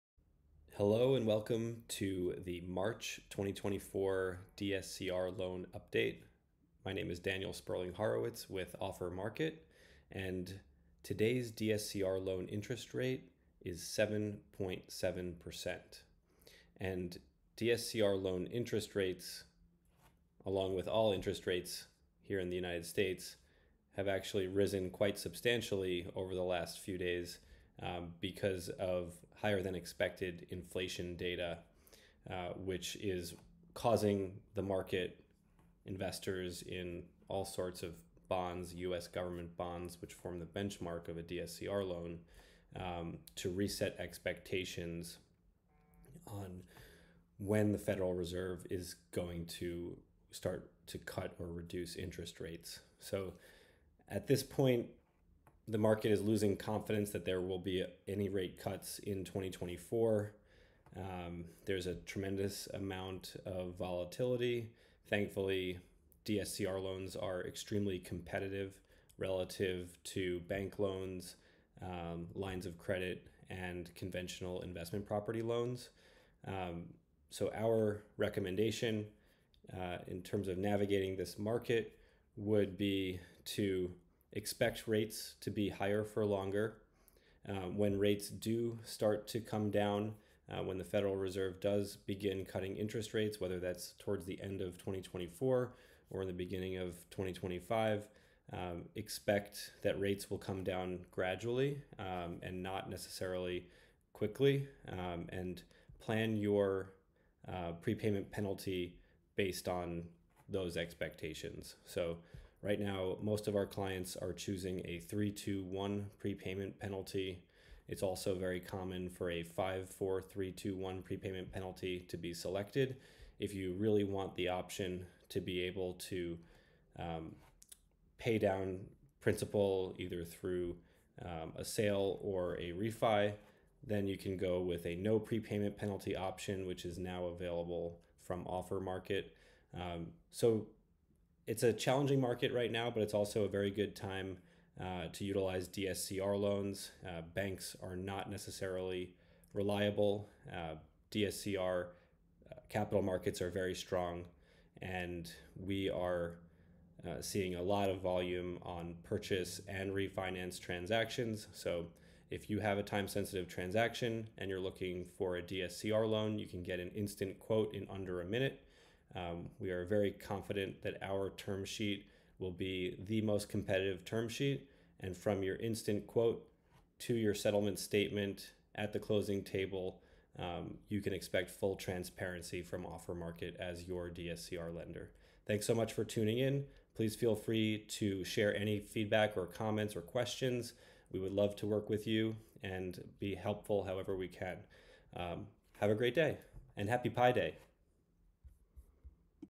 DSCR Loan Update - March 2024
00:00 Introduction
00:09 Today's DSCR loan interest rate
00:16 What's going on with interest rates?
01:17 How to navigate current interest rate environment
02:21 Good time to use DSCR loans?
02:41 Instant DSCR loan quote

In today's DSCR loan update, Daniel Sperling-Horowitz of OfferMarket explains what's going on with DSCR loan interest rates, and what you can expect as we move forward in 2024 and you plan your next purchase or refinance with a DSCR loan.

DSCR loan interest rates are +0.49% since February 1st, -0.67% since the October 2023 peak. DSCR loan rates are highly attractive relative to other financing options. Learn more about DSCR loans pros and cons

The market is digesting economic data and signals from members of the Board of Governors of Federal Reserve.

The recent rise in interest rates is due to an uptick in annual inflation from 3.1% to 3.2% in February, with inflation increasing +0.4% MoM, a 4.8% annualized rate and well off the Fed’s 2% target. This calls into question whether the Fed will actually start cutting rates in 2024.

It's important that you set conservative expectations -- do not be surprised if rates move higher from here in the near term and remain elevated into 2025. We expect continued volatility as the market digests economic data. When the Fed does begin cutting interest rates, unless there is an economic emergency, we expect it to be a gradual decline in rates.

Most borrowers are selecting a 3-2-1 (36 month) prepayment penalty which provides a competitive rate and no buydown, while also providing the optionality to cost effectively refinance or sell in the next few years.